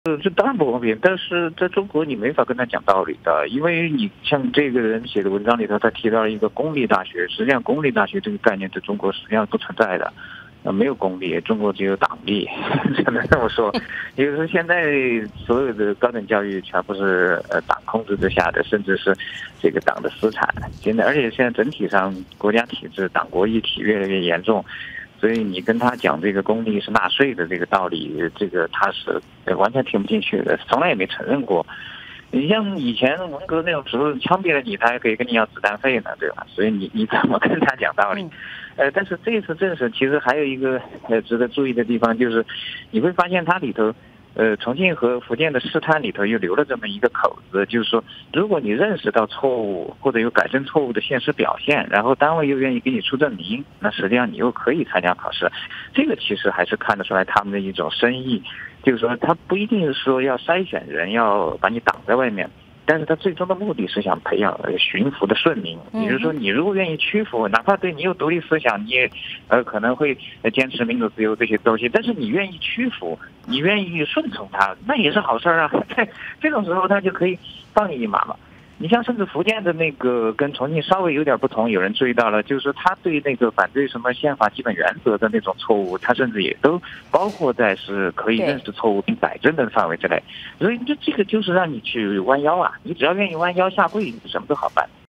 [吴戈：“政审”的最终目的是培养驯服的“顺民”]11/13
主持人：很多中国人都把高考看成是底层民众儿女改变命运的机会，而高考本身已是很不容易的一关，现在还要加上政审，您觉得这公平吗？结合《政审你大爷》这篇文章，谈谈你的看法。
独立时评人吴戈：这当然不公平，但在中国你没法跟官员讲道理。这篇文章中提到“公立大学”，但公立大学这个概念在中国其实是不存在的。在中国，没有公立，只有党立。现在中国所有的高等教育都在党控制之下，而且现在党国一体也越来越严重。你跟官员讲公立大学的钱是纳税人的钱，他们肯定完全听不进去。文革时，枪毙了你，还得跟你要子弹费呢，你怎么跟他讲道理？但这次政审还有一个值得注意的地方，重庆和福建的这次“试探”中留出了一个口子，就是如果你认识到错误，有改正错误的表现，学校还愿意给你出证明，那你还是可以参加考试。这当中还是能看出他们的深意，就是他们不一定是要筛选人，要把你挡在外面，但他们的最终目的是要培养驯服的顺民。哪怕你有独立思想，坚持民主自由这些东西，但如果你愿意屈服和顺从，那还是好事。只要愿意弯腰下跪，什么都好办。